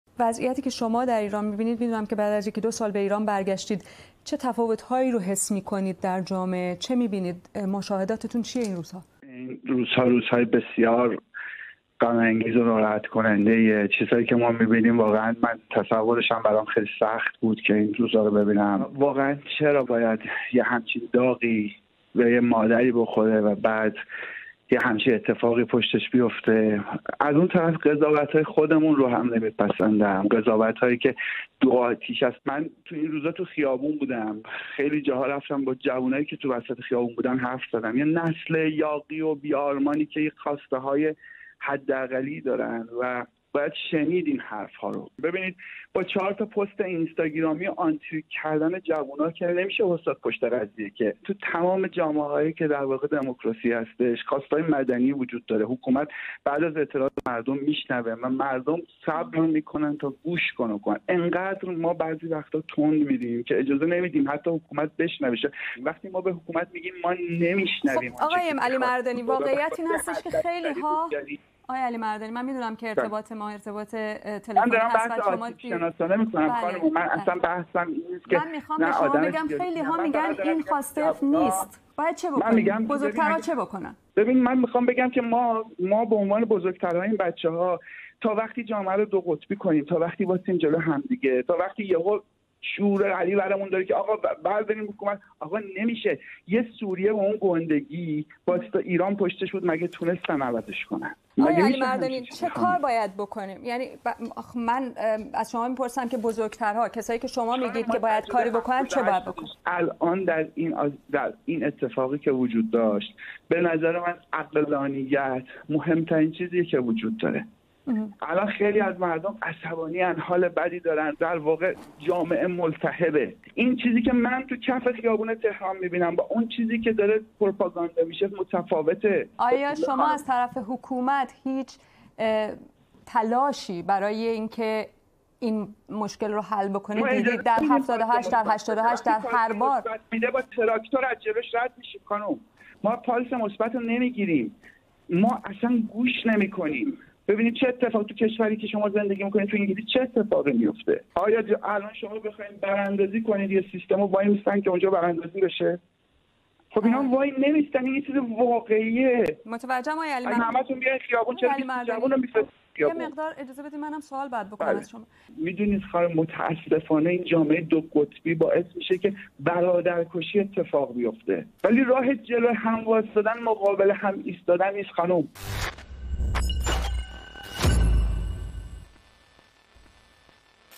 اظهارات طوفانی "هاتف علیمردانی"، کارگردان سینما خطاب به مجری شبکه سلطنتی بی‌بی‌سی
اگر بخواهید در انگلیس براندازی کنید سیستم حکومت می‌ایستد شما را نگاه می‌کند تا براندازی کنید؟!

⭕️ هاتف علیمردانی، تهیه‌کننده و کارگردان:

 ▪️آن‌چیزی که من در کف خیابان می‌بینم با آن چیزی که پروپاگاندا می‌شود متفاوت است؛ آنقدر تند می‌رویم که نمی‌گذاریم حکومت صدای برخی مردم را بشنود؛ کشور سوریه به آن بزرگی، یک ایران پشتش ایستاد نتوانستند حکومتش را تغییر دهند!